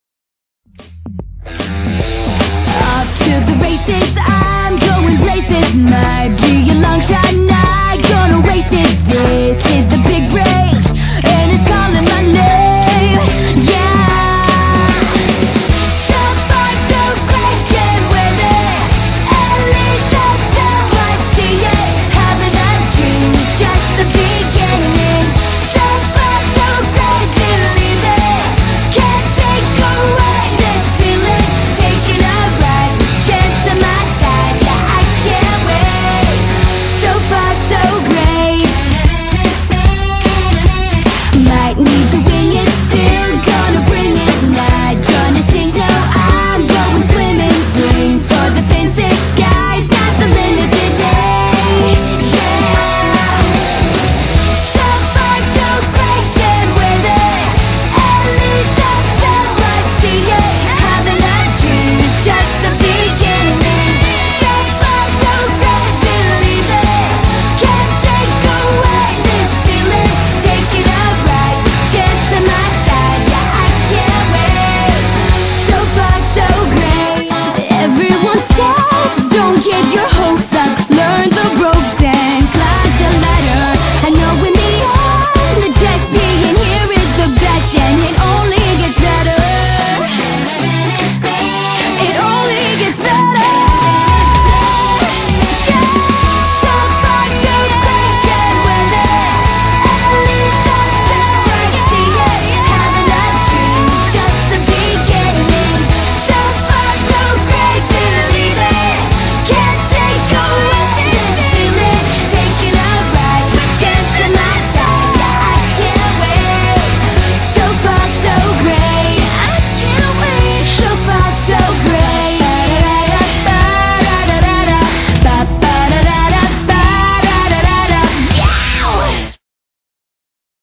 Sonny Entre Estrellas Antes y Después 2015
Actores de la serie Sonny Entre Estrellas en la actualidad